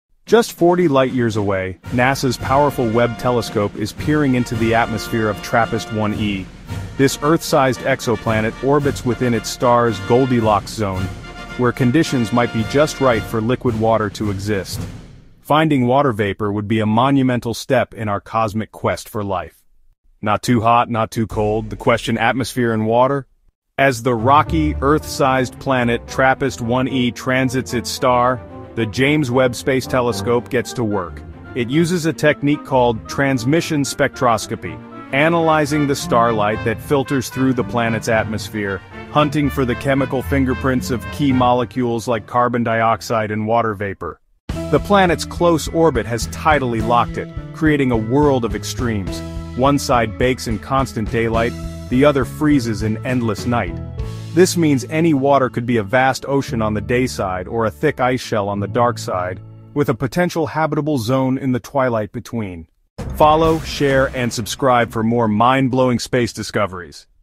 NASA's Webb Telescope DISCOVERS New Earth-like Planet in Record Time!
NASA's Webb Telescope has made a groundbreaking discovery - a new Earth-like planet, all in record time! Explore the wonders of the universe with NASA's latest find.
NASA’s James Webb Space Telescope is zooming in on TRAPPIST-1 e, an Earth-sized exoplanet in the Goldilocks Zone!"